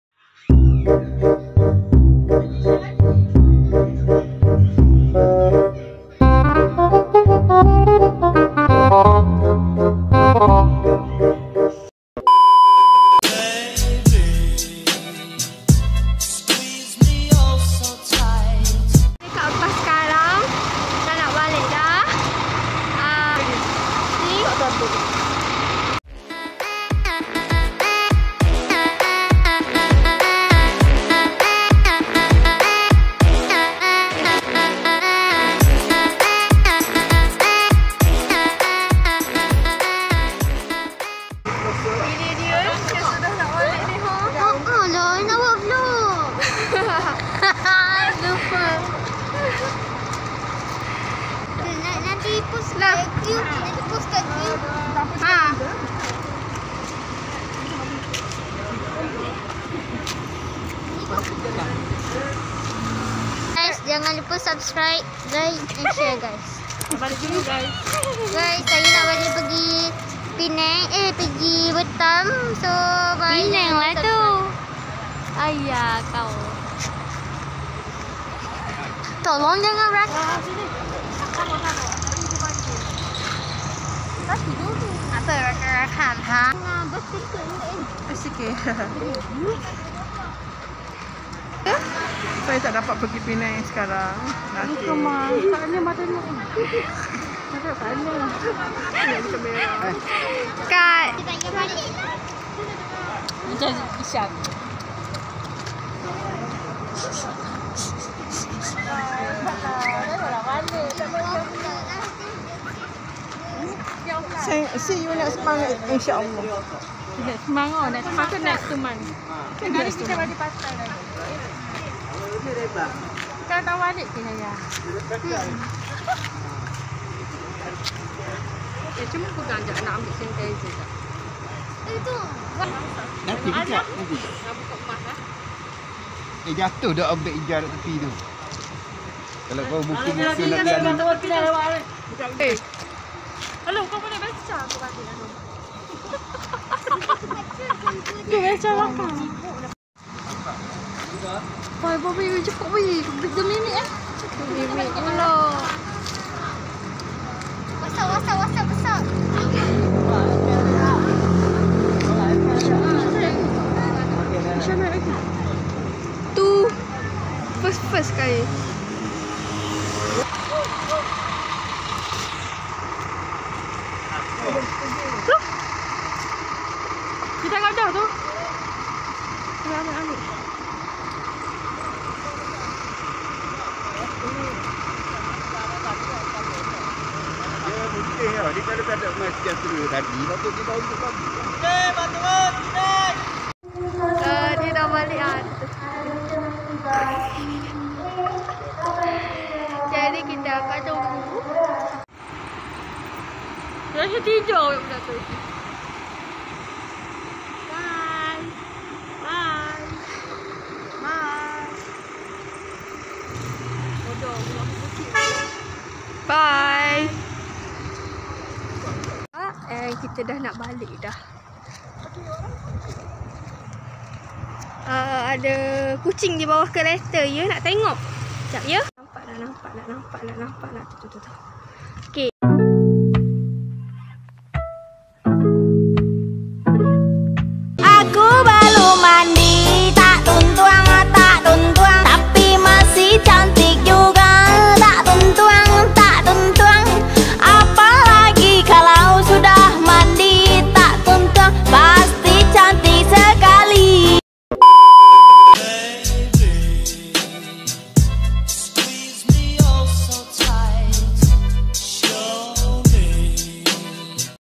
[30/10/21] Eshal Buat TIKTOK + Balik Penang [Short Vlog]
Ok video kali ni takde caption